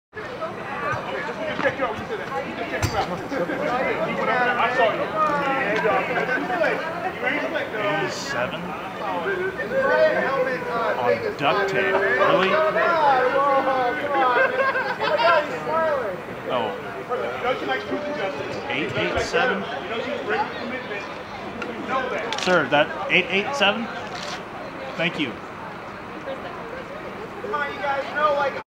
ACSO Deputy #1887 Name Covered With Duct Tape
Alameda County Sheriff's Deputy #1887 had his name covered with duct tape and a badge number written on it with a sharpie. NOT LEGAL, ACSO! January 28th, 2012, near the YMCA where ~409 peaceful protestors were illegally kettled and arrested.